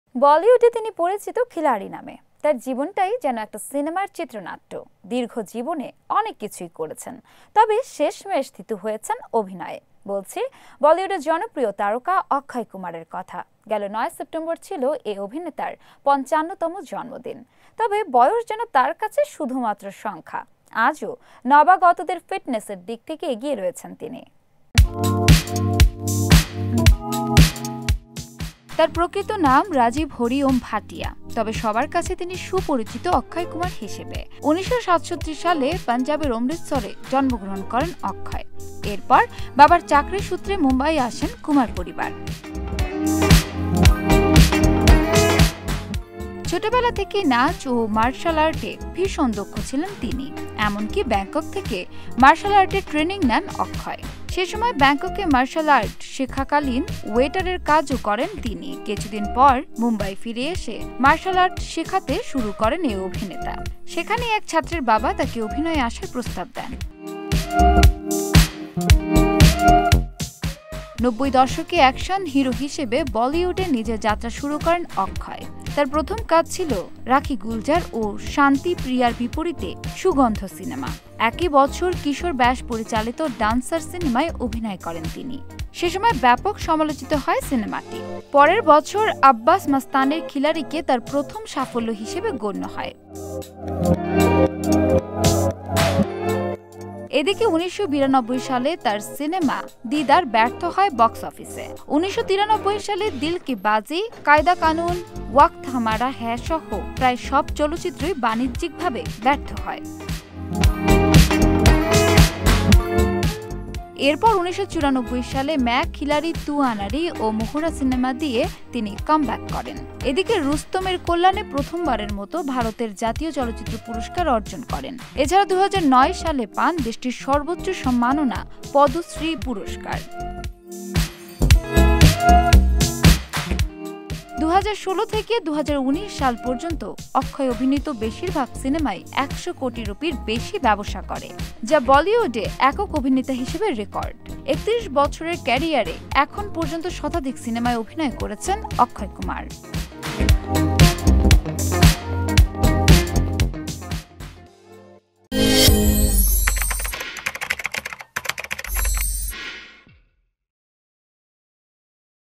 মার্শাল আর্টের শিক্ষক থেকে অভিনেতা। Akshay। Bijoy TV
তার প্রকৃত নাম রাজিব হরি ওম ভাটিয়া। তবে, সবার কাছে তিনি সুপরিচিত অক্ষয় কুমার হিসেবে। ১৯৬৭ সালে পাঞ্জাবের অমৃতসরে জন্মগ্রহণ করেন অক্ষয়। এরপর বাবার চাকরি সূত্রে মুম্বাই আসেন কুমার পরিবার।
ছোটবেলা থেকেই নাচ ও মার্শাল আর্টে ভীষণ দক্ষ ছিলেন তিনি। এমনকি ব্যাংকক থেকে মার্শাল আর্টের ট্রেনিং নেন অক্ষয়। সেসময় ব্যাংককে মার্শাল আর্ট শেখাকালীন ওয়েটারেরও কাজ করেন তিনি। কিছুদিন পর মুম্বাই ফিরে এসে মার্শাল আর্ট শেখাতে শুরু করেন এ অভিনেতা। সেখানেই এক ছাত্রের বাবা তাকে অভিনয়ে আসার প্রস্তাব দেন।
নব্বই দশকে অ্যাকশন হিরো হিসেবে বলিউডে নিজের যাত্রা শুরু করেন অক্ষয়। তার প্রথম কাজ ছিল রাখী গুলজার ও শান্তিপ্রিয়ার বিপরীতে ‘সুগন্ধ’ সিনেমা। একই বছর কিশোর ব্যস পরিচালিত ‘ড্যান্সার’ সিনেমায় অভিনয় করেন তিনি। সে সময় ব্যাপক সমালোচিত হয় সিনেমাটি। পরের বছর আব্বাস-মাস্তানের ‘খিলাড়ি’ কে তার প্রথম সাফল্য হিসেবে গণ্য হয়।
এদিকে, ১৯৯২ সালে তার সিনেমা ‘দিদার’ ব্যর্থ হয় বক্সঅফিসে। ১৯৯৩ সালে দিল কী বাজী, কায়দাকানুন, ওয়াক্ত হামারা হ্যায়সহ প্রায় সব চলচ্চিত্রই বাণিজ্যিকভাবে ব্যর্থ হয়।
এরপর ১৯৯৪ সালে ম্যাঁয় খিলাড়ি তু আনাড়ি ও মোহরা সিনেমা দিয়ে তিনি কামব্যাক করেন। এদিকে, ‘রুস্তম’-এর কল্যাণে প্রথমবারের মতো ভারতের জাতীয় চলচ্চিত্র পুরস্কার অর্জন করেন। এছাড়া ২০০৯ সালে পান দেশটির সর্বোচ্চ সম্মাননা পদ্মশ্রী পুরস্কার।